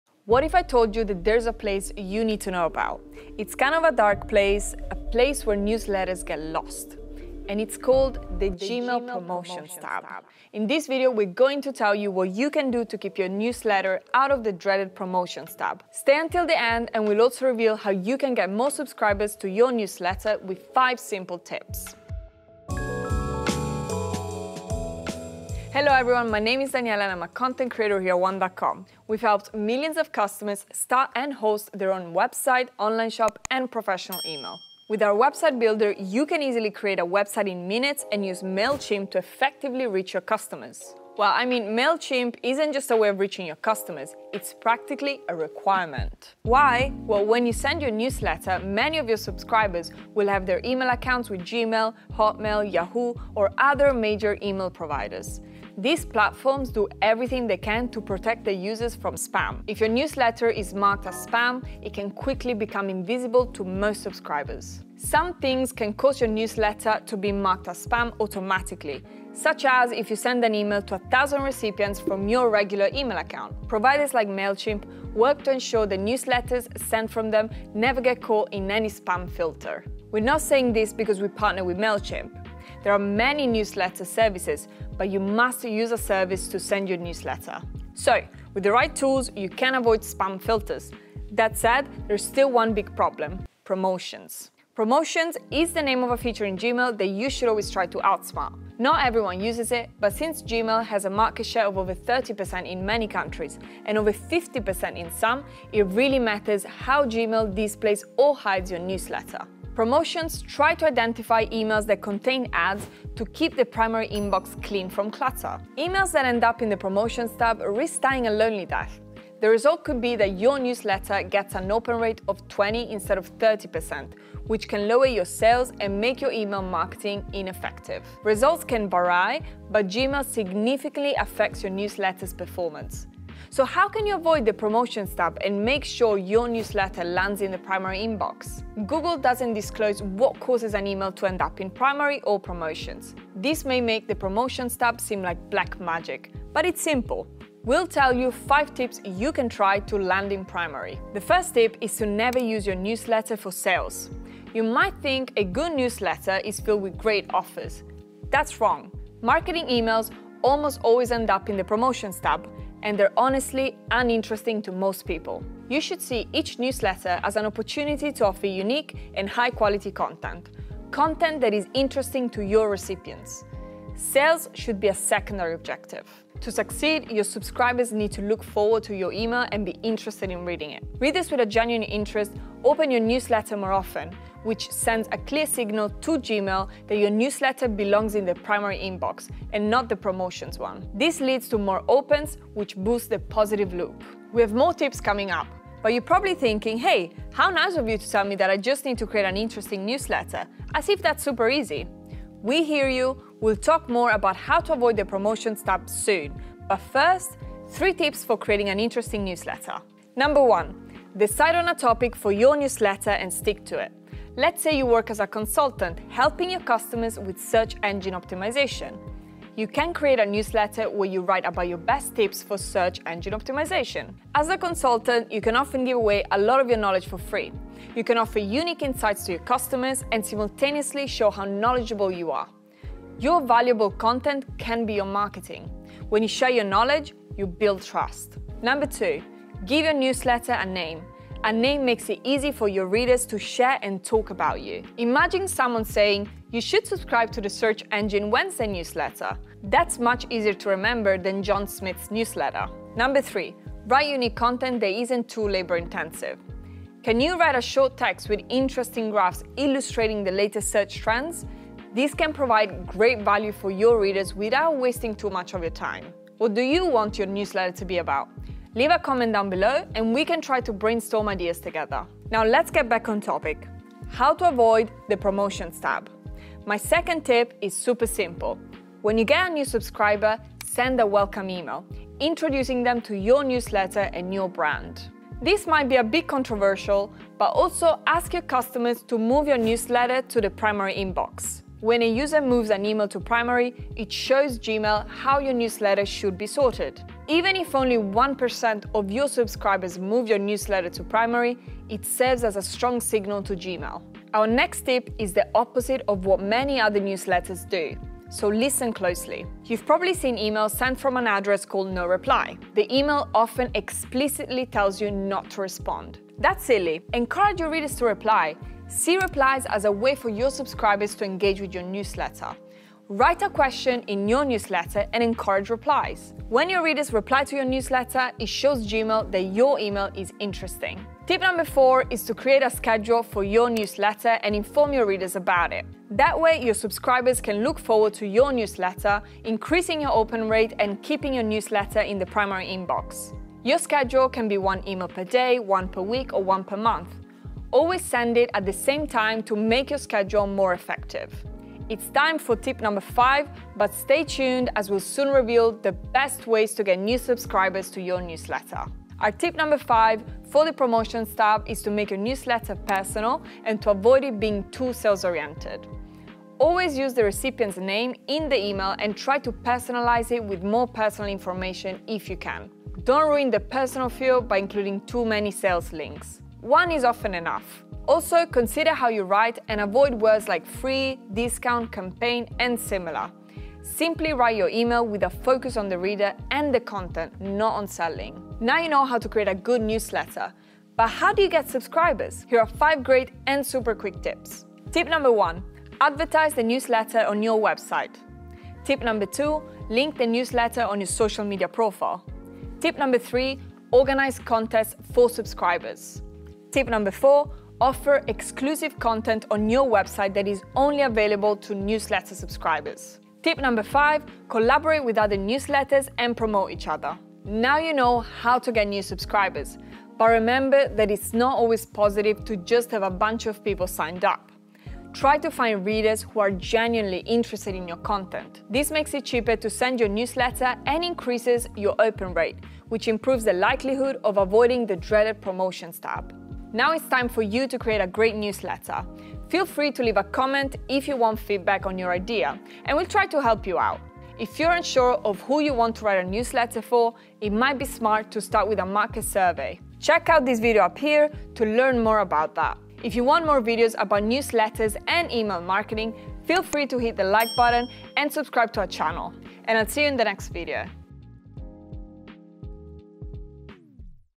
The reason many never open your email newsletter
Struggling with low open rates on your email newsletter? In this video, we dive into the top reasons why people might be ignoring your emails and how to fix it!

Twitter (x):  @onecom

02:30 Tip 1
04:36 Tip 2
05:18 Tip 3
05:31 Tip 4
05:53 Tip 5